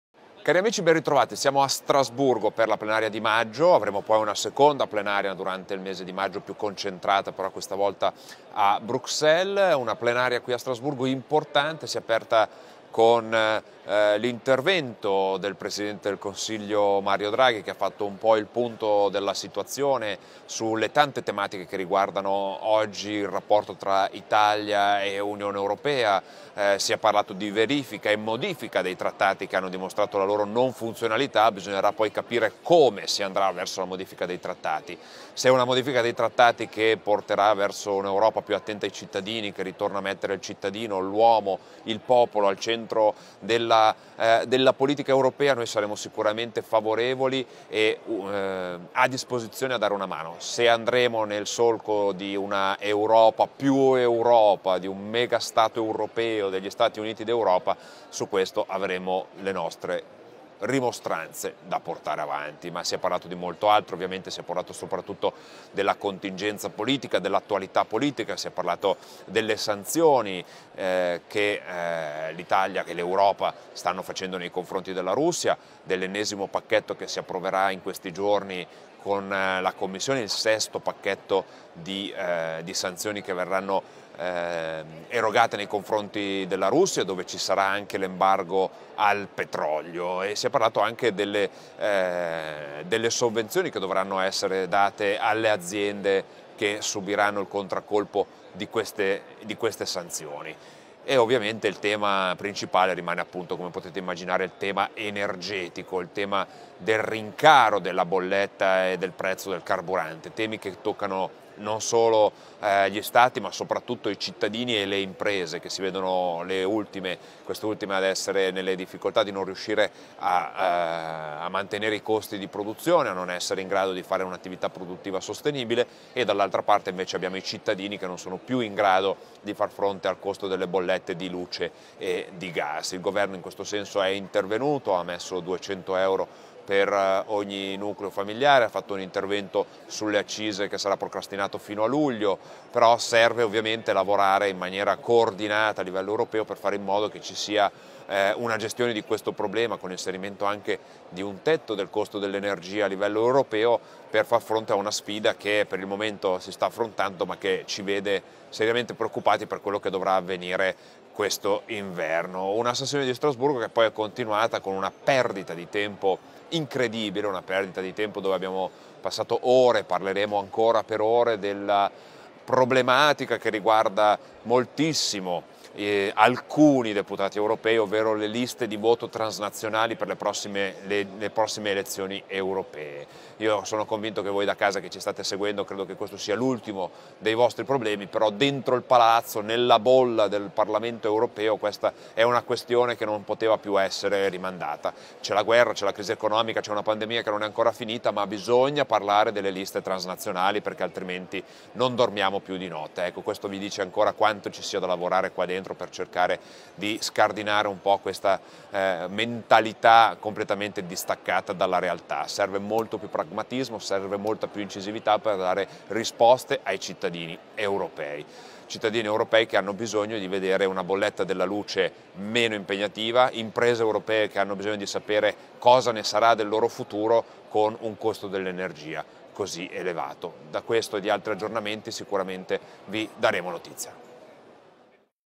ALESSANDRO PANZA: "COMMENTIAMO INSIEME LA PLENARIA DEL PARLAMENTO EUROPEO DI STRASBURGO"
(Agenzia Italia News) L' EUROPARLAMENTARE ALESSANDRO PANZA: "COMMENTIAMO INSIEME LA PLENARIA DEL PARLAMENTO EUROPEO DI STRASBURGO"